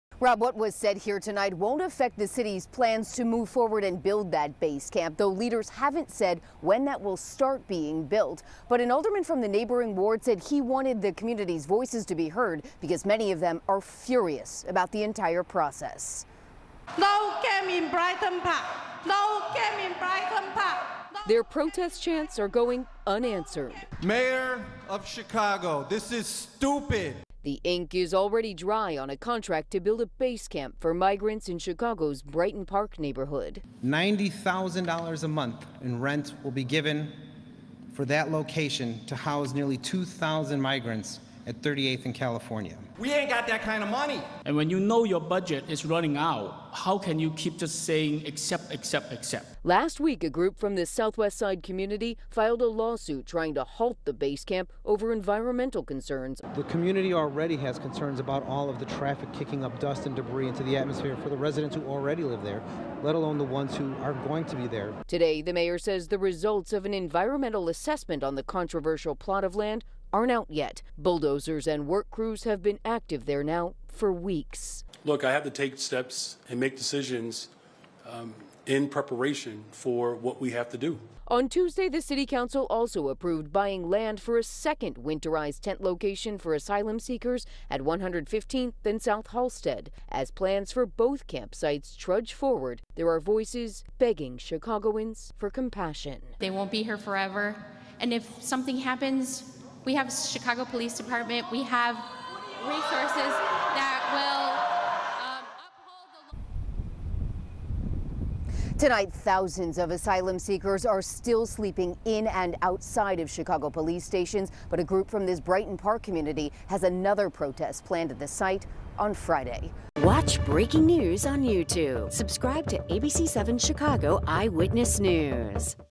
Residents concerned as plans for Chicago migrant camp move forward
Brighton Park residents expressed their concerns as plans to house Chicago migrants at 38th and California move forward.